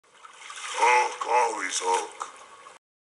Marvel: Hulk Always Hulk
here is a video of hulk saying this line, so i hope you all really enjoy it